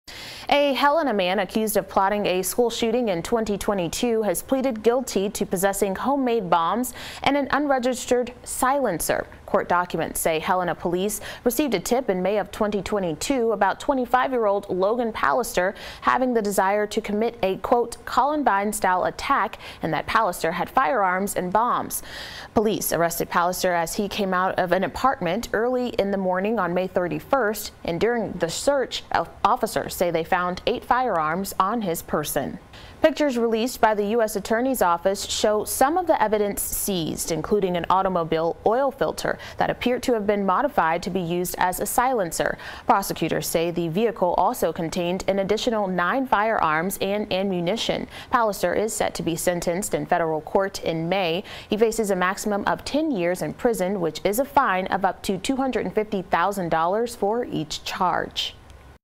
Man accused of threatening Helena school pleads guilty to possessing homemade explosives
A Helena man accused of making threats against Helena Public Schools has admitted to possessing homemade bombs.